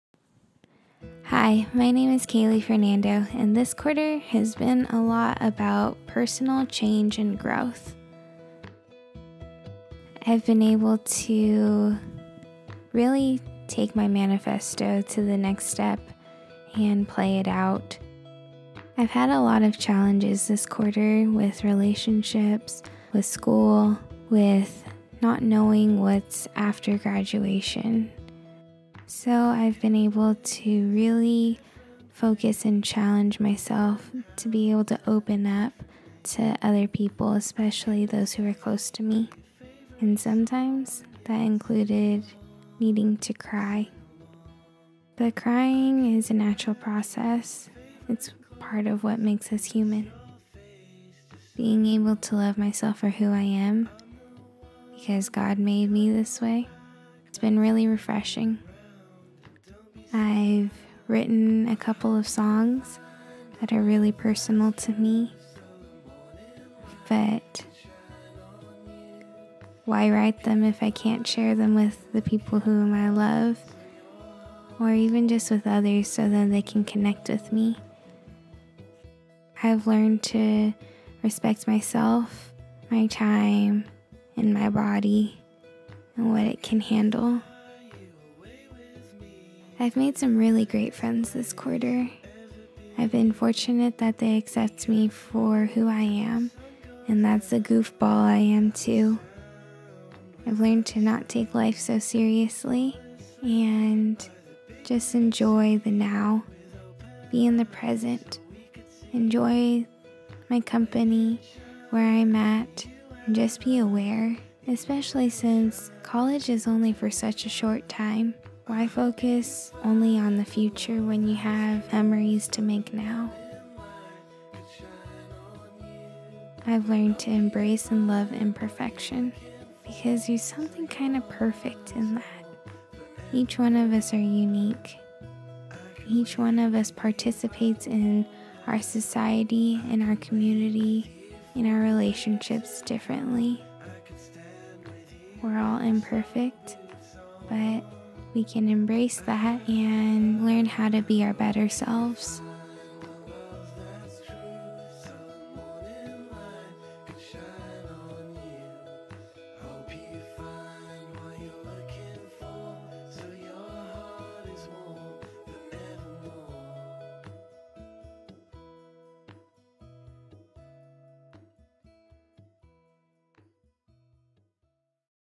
Change & Growth by Keili Fernando
Taking the next step in implementing my manifesto into my life.

Documentation & Reflection:

An Executive Summary/Abstract: I chose to do an online video project because I wanted to challenge myself in technical and artistic processes while using technological advancements. My manifesto was a collage of photos with text. I wanted to push the creativity forward by making a video that would give a brief abstract on my process of implementing some of my goals into my life. For each of my points, I did the following:

1. Move more: I joined a dodgeball team and a yoga class this quarter.
2. Don't be afraid to cry: This quarter has been particularly rough, so keeping a sunny disposition all the time has been incredibly difficult. However, I have found a couple close friends who I feel comfortable opening up to. I have also contacted my parents about hardships that I have been going through. I have developed a bravery to share my vulnerabilities with them.
3. Stop comparing: When it comes to making art, I become very insecure because I don’t know how people will react to it, especially films. However, I have learned to make my art for me as part of self-expression, starting with the making of this video.
4. Delegate & share: I hold 3 leadership positions in student organizations. I am the Director of External Affairs for Triton Television, the Managing Director of Company 157, and the Webmaster/Community Advisor for the Undergraduate Communication Society. With these positions, I lead groups of members in committees. I have received feedback from members that I have done a good job of giving them roles and tasks. I have never held so many positions at the same time, so this was a challenge to decide who should perform which tasks at hand. I have also sought out ways to make delegating easier. Social media has been a great help with this.
5. Sing & be open: I joined a singing class this quarter as well as wrote a couple of new songs. I performed my original songs publicly five times. These songs are very personal to me, so being able to share them with others was new, refreshing, and expressive.
6. Eat more natural foods: I have been cooking a lot more and buying whole foods rather than preserved snacks. However, I do fall short in that I give into ice cream and desserts. I have also created a Pinterest board of healthy recipes.
7. Understand my limitations: I realized that I put a lot of effort into things that I do not necessarily enjoy and receive little to no reward, and I end up getting hurt in the process. For example, I have served many hard hours helping people who do not show any gratitude for my time and efforts. Therefore, I feel unappreciated, disrespected, and extremely tired. I became sick after putting in “too much” effort into projects; I decided that it is not worth the time or energy if there is little reward. I do consider my own enjoyment as a reward though.
8. Be a goofball: I am trying to not take life so seriously. This has been a challenge because graduation is just around the corner. When I am comfortable with the people I am with, I tend to be silly and loose.
9. Enjoy the now: With all the recent focus on “what am I going to do after graduation,” it is a challenge to focus on the memories being made in the present. My friends who are younger than me often help me think about and enjoy the present moments. In yoga, the teacher discusses a lot about living in the moment during meditation.
10. Embrace & love imperfection: This quarter, I wanted to take everything in stride and take time to appreciate myself and things around me.

Course concepts covered/put to work:
Bennett
Theme of Ideas & Letters
Gates
Soep

Evidence: I interviewed one of my closest friends and my boyfriend. I collected my food receipts to keep my diet on track. I wrote a song and performed it publicly twice. I also did a lot of self-reflection by writing in my journal--as shown in the video.

Reflection: At first, I was very excited to get started on implementing my manifesto to my life. I went full speed. However, after a week, it became harder, and I needed to be more disciplined with myself. If I was given more time, I would probably have more confidence in executing my decisions and actions while performing my manifesto. I would focus on doing each of the points with greater commitment and keep applying tactics until they feel completely normal. My stubbornness and willingness to take on too many tasks were initially invisible, but they became visible. I interviewed one of my closest friends, and he gave me great insight to how I had changed (mainly for the better). I also interviewed my boyfriend a couple of times throughout the quarter. He revealed to me that I have a great amount of patience and selflessness when it comes to working with other people—including himself.